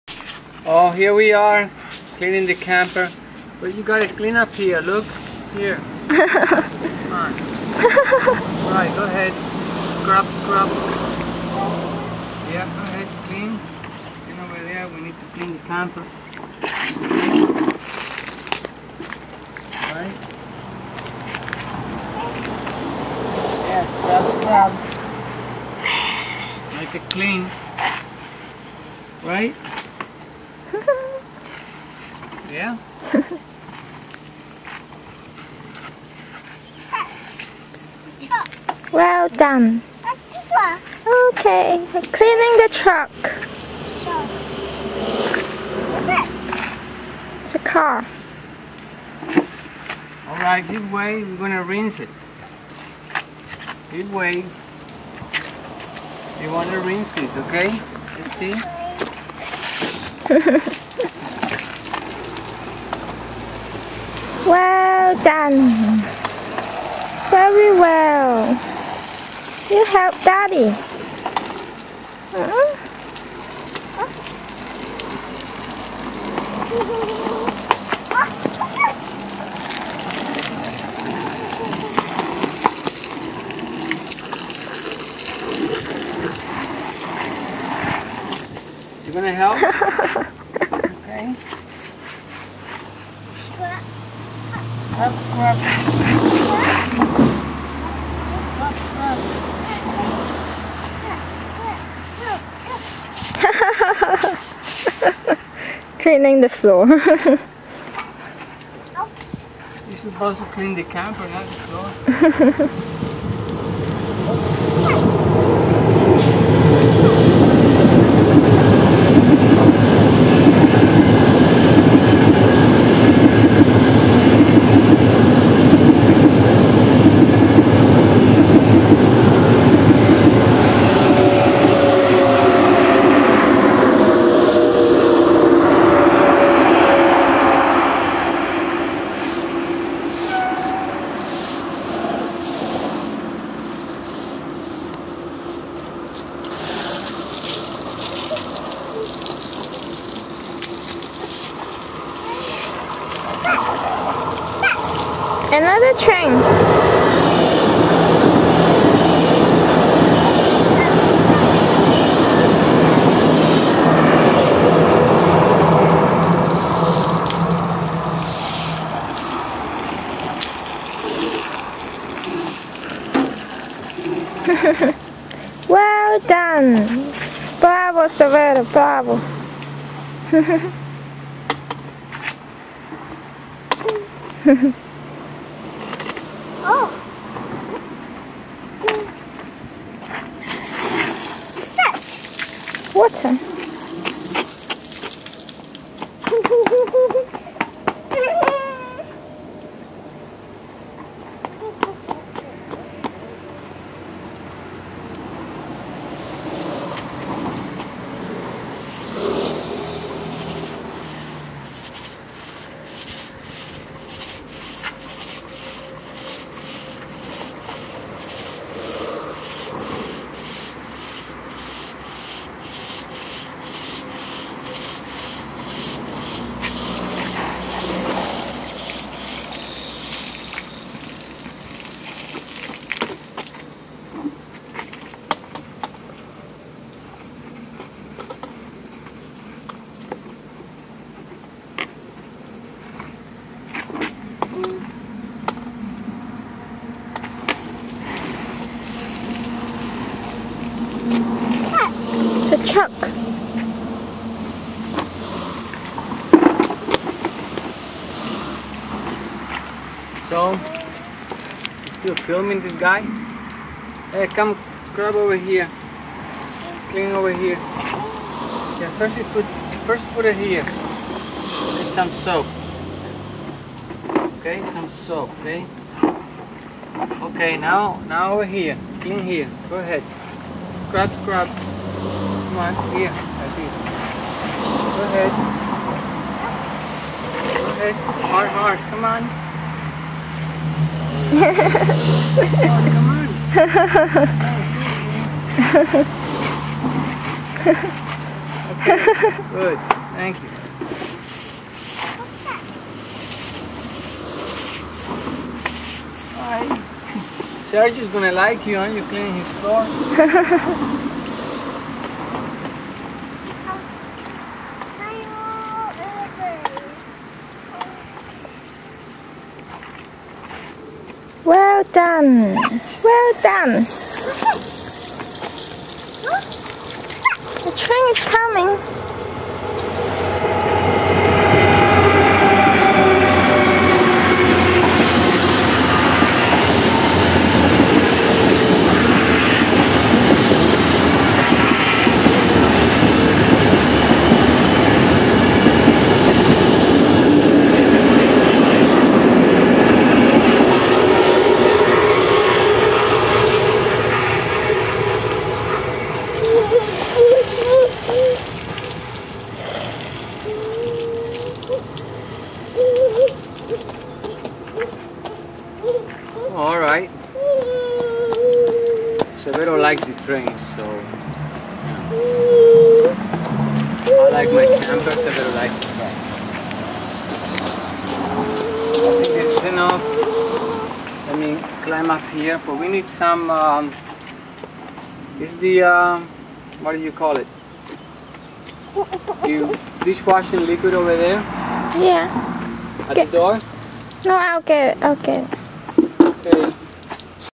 Star Trek Paulo Severo Hernandez I can Help
Here we are in Florence, the motor home needed washing, Severo was always ready to help